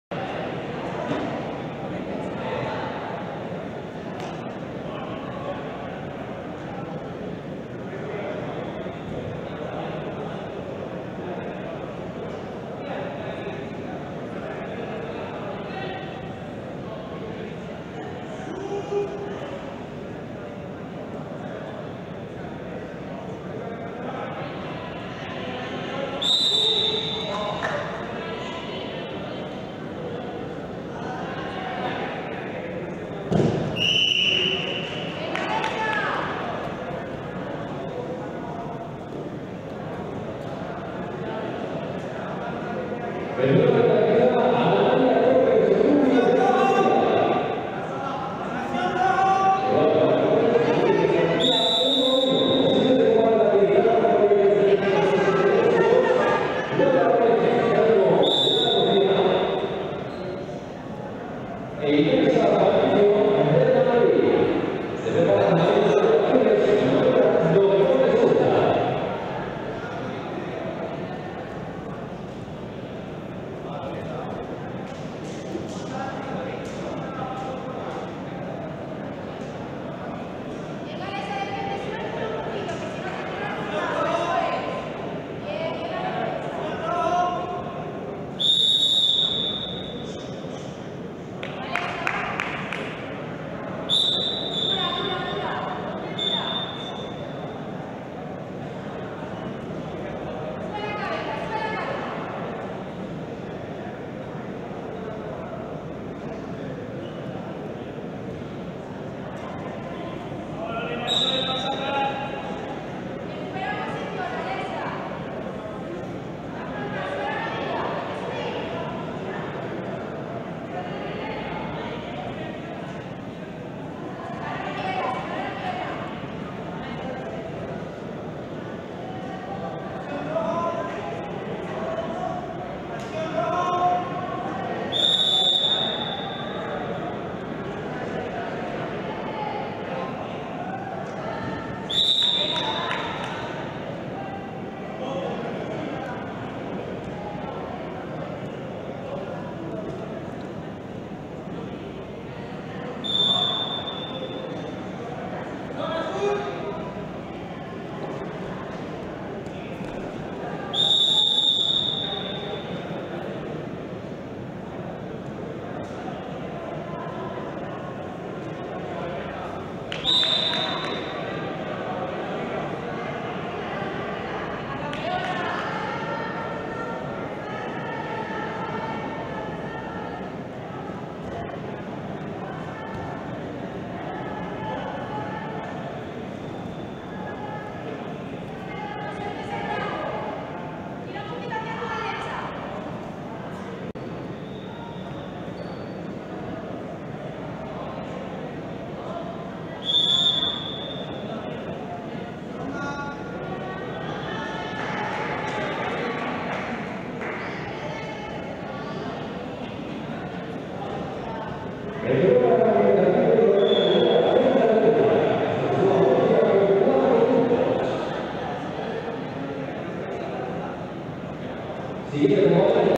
63º Combate - LF 76KG - LERA CELDA, LORENA (MUR) VS ACEBRON HOLGADO, CARLOS (MAD)
3º y 4º puesto - Campeonato de España de Lucha por Autonomías 2021 - MURCIA VS MADRID 2